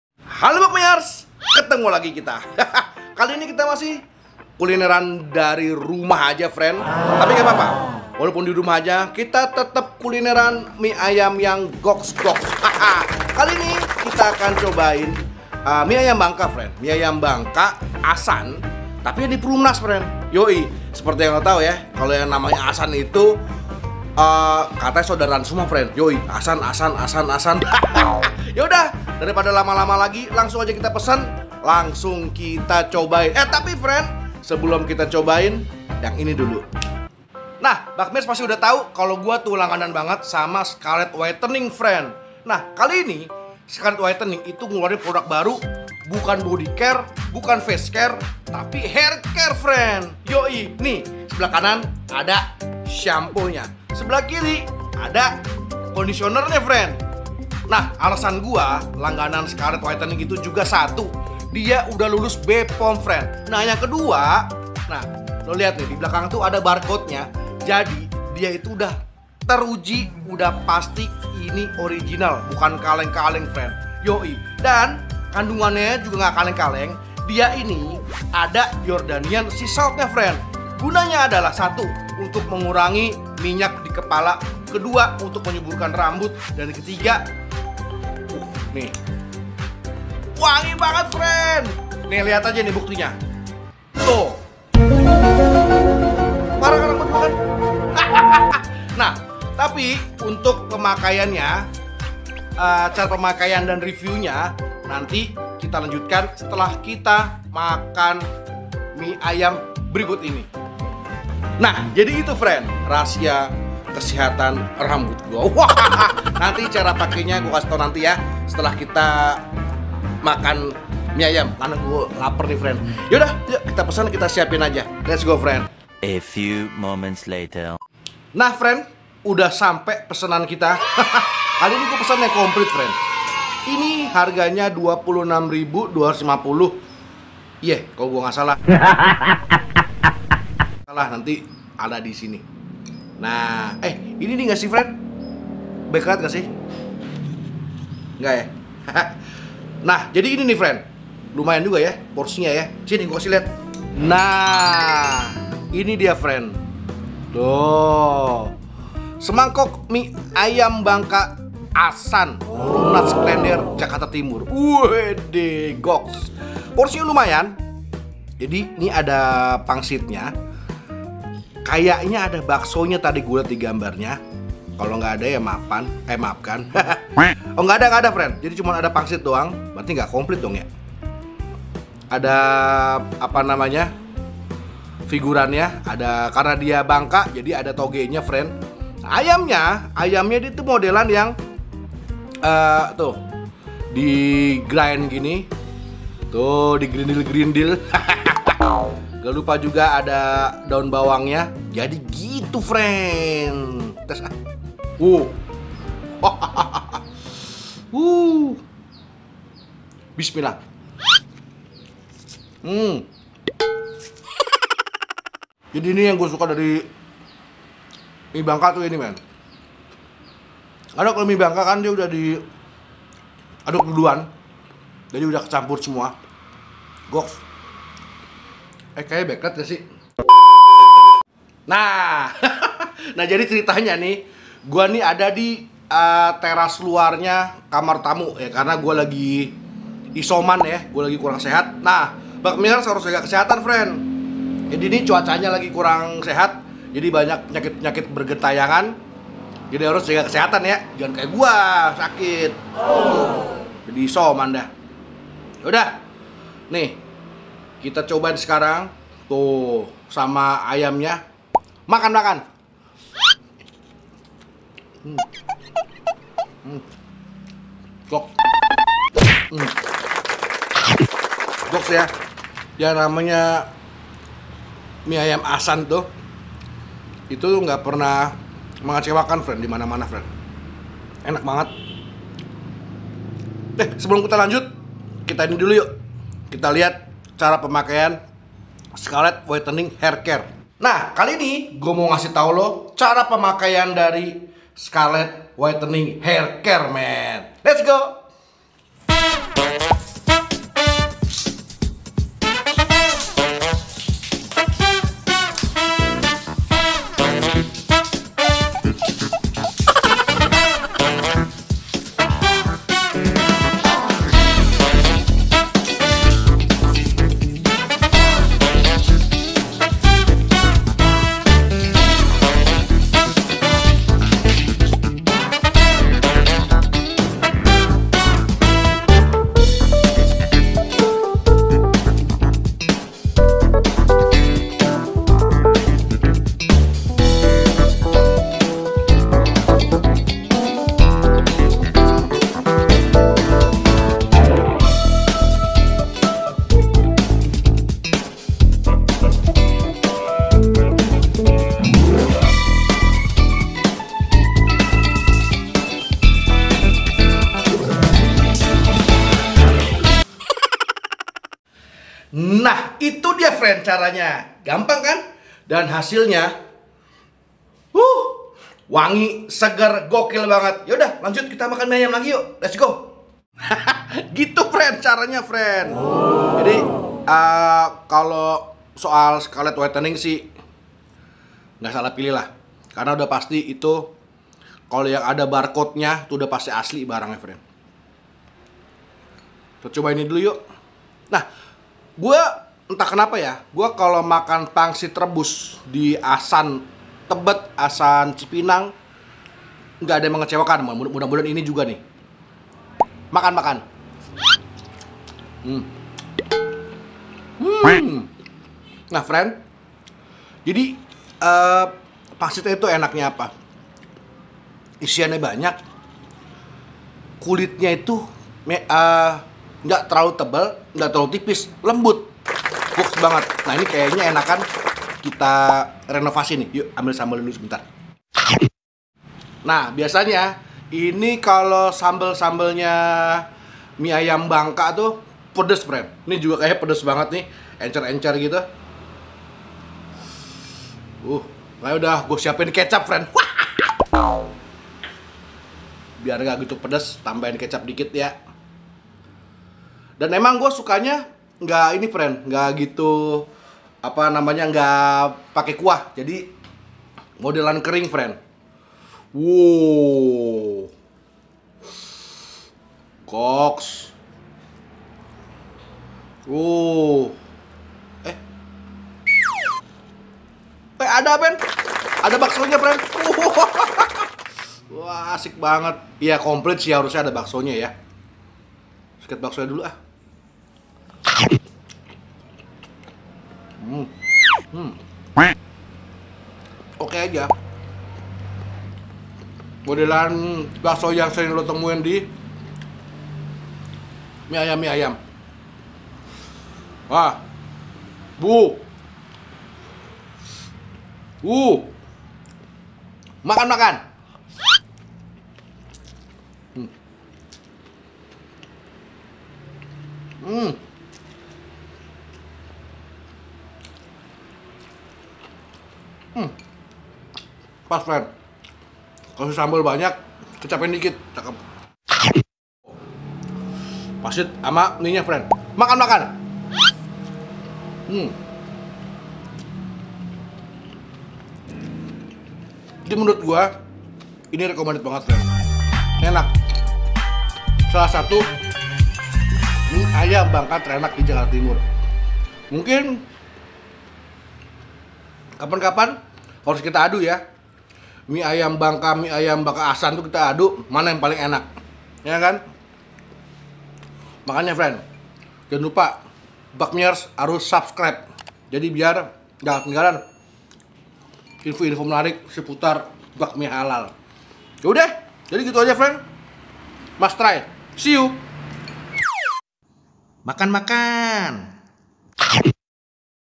MIE AYAM BANGKA INI ENAK BANGET!! KULINER DI JAKARTA TIMUR
Mie ayam bangka ASAN pasti udah banyak yg tau ni,nah kuliner jakarta timur kali ini kita akan makan  mie ayam bangka ASAN yg di perumnas klender Jakarta timur friend!! mie ayam yg satu ini rating google nya juga bagus ni,ga ada yg bilang ga enak..
Bakmiers harus coba!!

📍 MIE AYAM BANGKA ASAN PERUMNAS
Jl. Teratai Putih Raya No.5d, RW.4, Malaka Sari, Kec. Duren Sawit, Daerah Khusus Ibukota Jakarta 13460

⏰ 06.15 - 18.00

100% halal